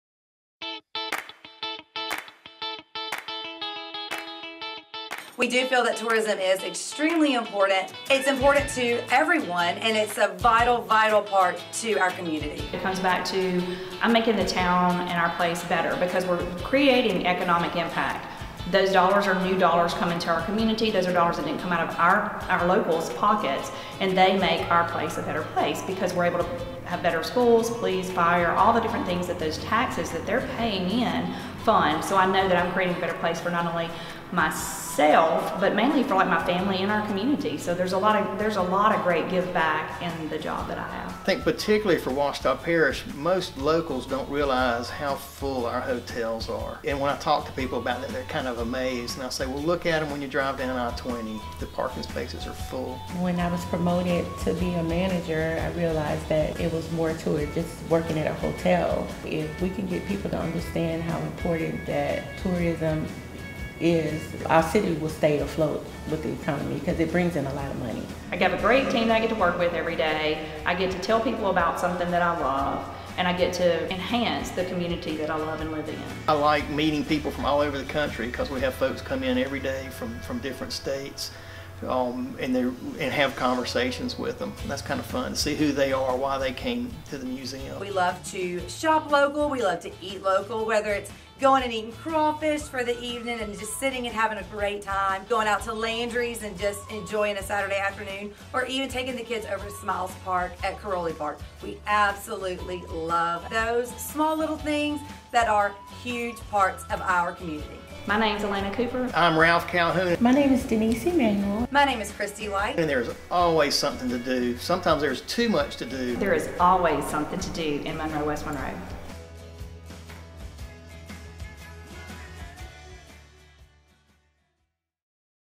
Faces of Tourism 1
It's Tourism Awareness Week! We gathered just a few of our friends to talk tourism in Ouachita Parish.